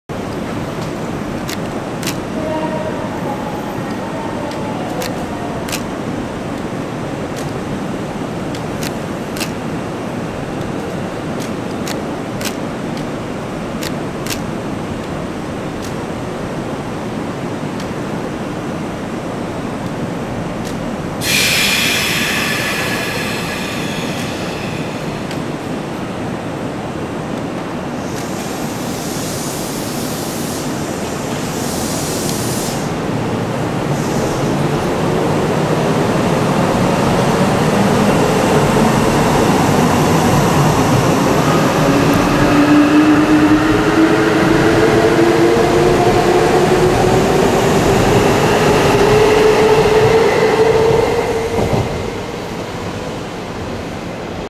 臨時えのしま50号新宿ゆき（臨時幕/LSE旧塗装） 大和駅発車
小田急電鉄では夏季行楽シーズンの多客対応のため、臨時特急ロマンスカーえのしま50・52号を藤沢→新宿間で運転しています。
臨時特急のうち1本はLSEの運用で、普段は使われない「臨時」幕が表示されます。

2012/08/15 小田急江ノ島線大和駅